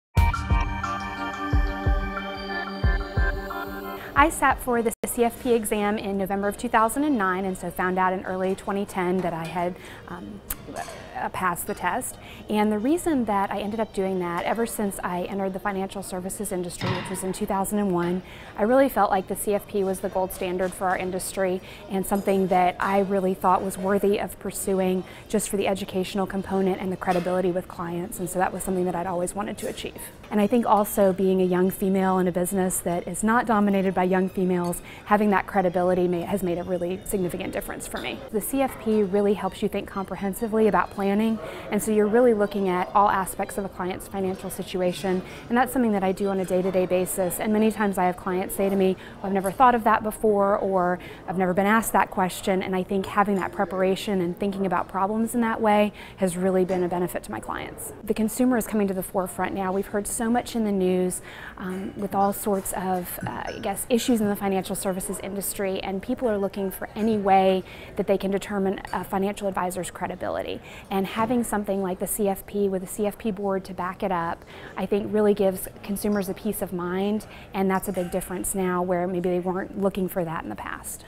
Clarissa Hobson, CFP® on CFP® Certification
Clarissa Hobson, CFP®, Senior Financial Planning Advisor, Carnick & Kubik is interviewed about the credibility she gained by becoming a CFP® professional and the benefits of CFP® certification -- the highest standard of personal financial planning.